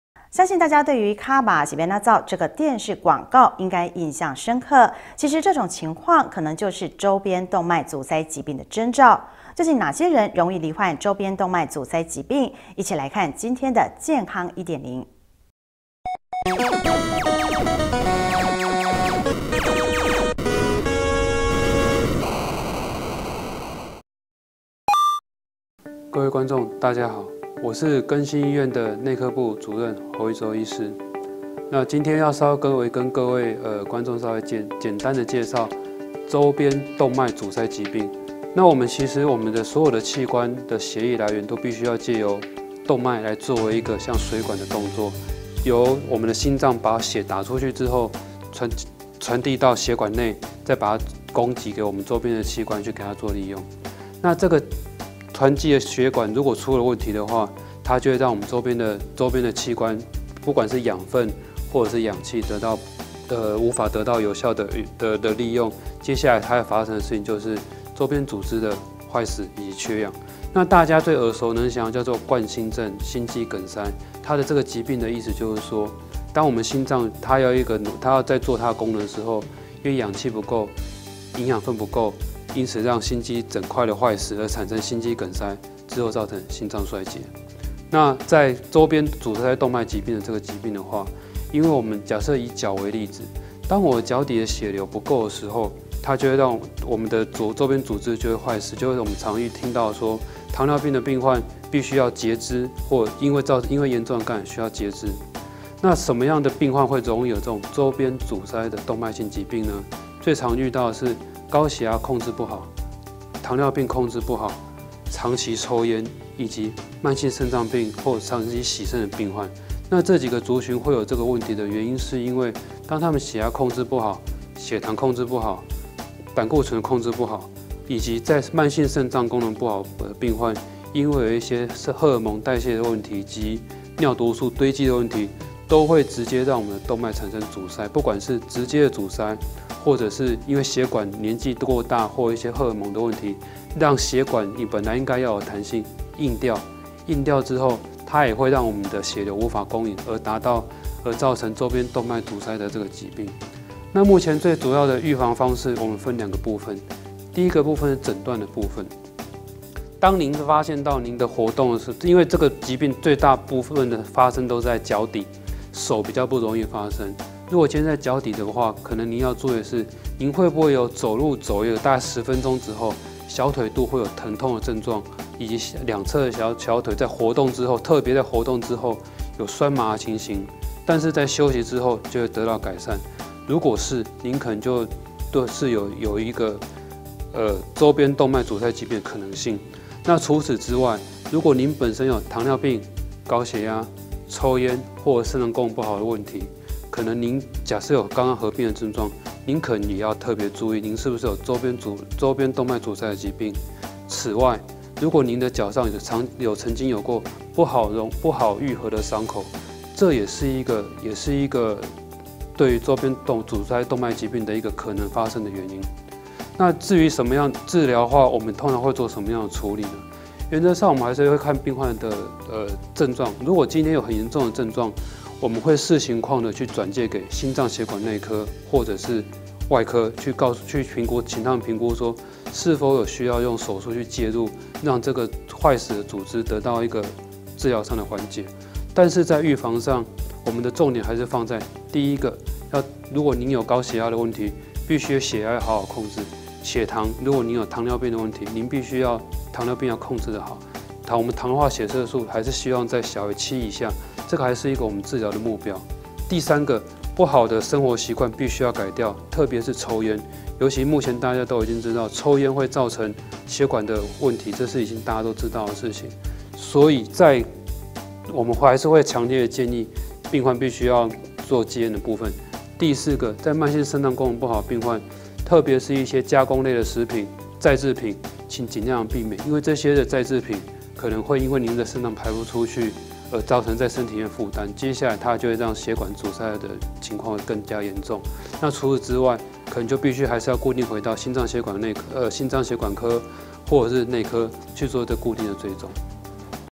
1081203【大新店地方新聞】健康醫點靈 週邊動脈阻塞疾病
大家對於『腳麻了是要怎麼逃』這個電視廣告應該印象深刻！其實這種情況可能就是週邊動脈阻塞疾病的徵兆，究竟哪些人容易罹患週邊動脈阻塞疾病，一起來看今天的健康一點靈。